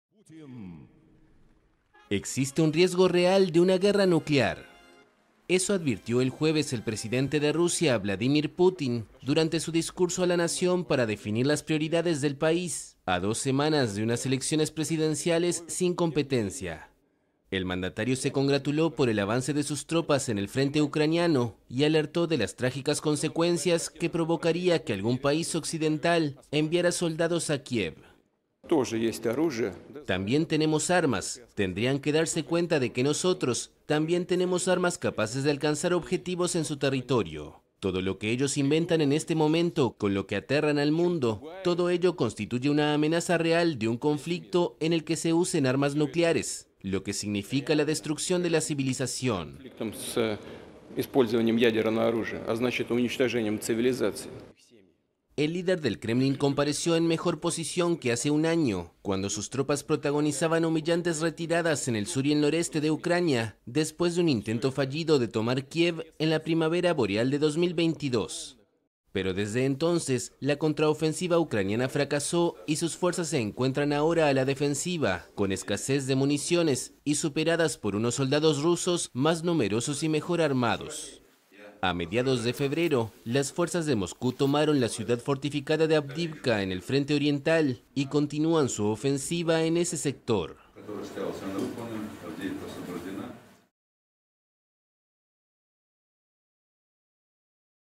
RUSIA I Putin advirtió a los países occidentales del riesgo "real" de guerra nuclear
El presidente de Rusia, Vladimir Putin, advirtió el jueves a las potencias occidentales del riesgo "real" de una guerra nuclear en caso de escalada en el conflicto en Ucrania, en su discurso a la Nación para definir las prioridades del país, a dos semanas de unas elecciones presidenciales sin competencia.